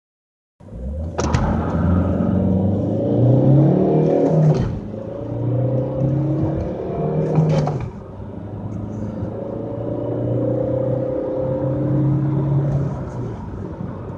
Fiat Bravo II 1.9 120PS Przyspieszenie Acceleration
przyspieszenie Fiata Bravo 1.9 120Koni , fabryka 10,1s
Acceleration Fiat Bravo 1.9 120PS , stock 10,1s